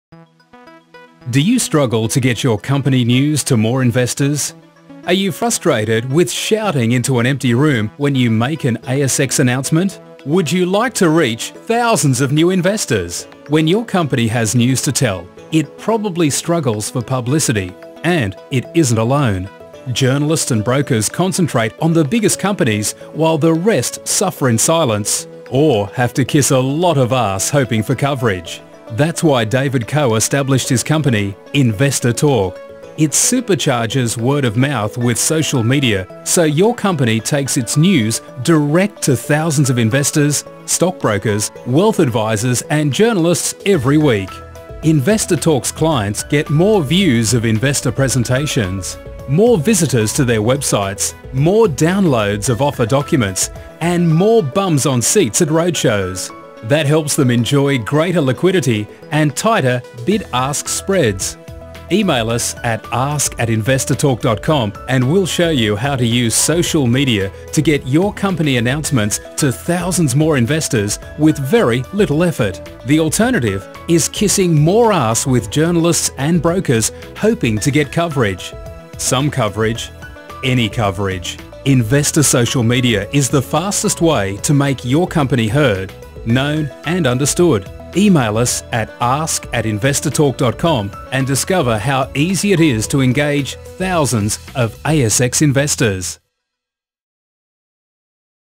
The easy way to engage new shareholders
Companies that are floating or raising capital need investors who recognise the true value of their shares. But it has never been harder to reach investors.

Even companies that simply want their shares priced fairly can struggle to engage new shareholders.

The solution is to harness the viral power of social media and engage investors direct.